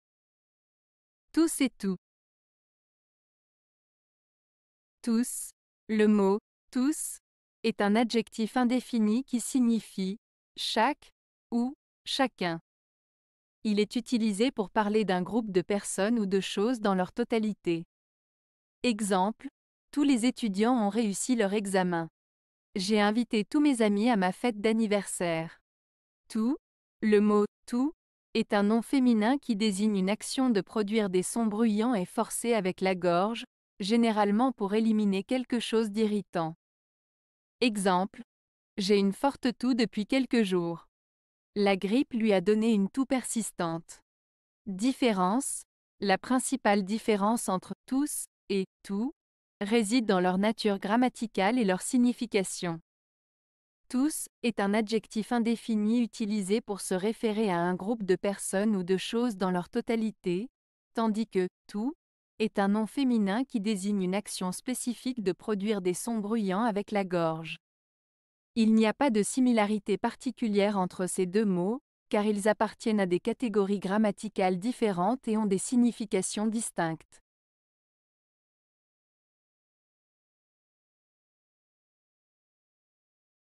Tous et Toux (Une Lettre)
Tous:
Le mot "tous" est un adjectif indéfini qui signifie "chaque" ou "chacun". Il est utilisé pour parler d'un groupe de personnes ou de choses dans leur totalité.

Exemples:
- Tous les étudiants ont réussi leur examen.
- J'ai invité tous mes amis à ma fête d'anniversaire.

Toux:
Le mot "toux" est un nom féminin qui désigne une action de produire des sons bruyants et forcés avec la gorge, généralement pour éliminer quelque chose d'irritant.

Exemples:
- J'ai une forte toux depuis quelques jours.
- La grippe lui a donné une toux persistante.

Différence:

La principale différence entre "tous" et "toux" réside dans leur nature grammaticale et leur signification. "Tous" est un adjectif indéfini utilisé pour se référer à un groupe de personnes ou de choses dans leur totalité, tandis que "toux" est un nom féminin qui désigne une action spécifique de produire des sons bruyants avec la gorge. Il n'y a pas de similarités particulières entre ces deux mots, car ils appartiennent à des catégories grammaticales différentes et ont des significations distinctes.